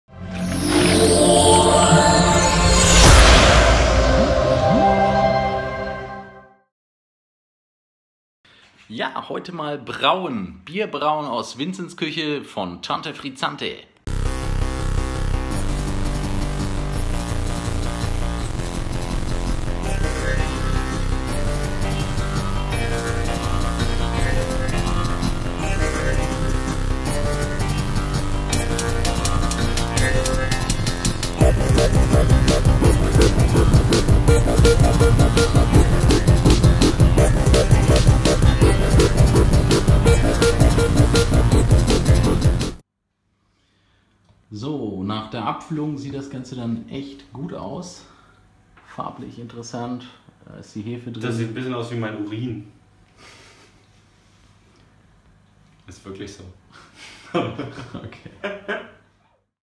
Neukölln trinkt - Blog Episode 3 - Craft Beer selber brauen.
Jan und Vincent brauen Ein leckeres IPA mit der Braubox.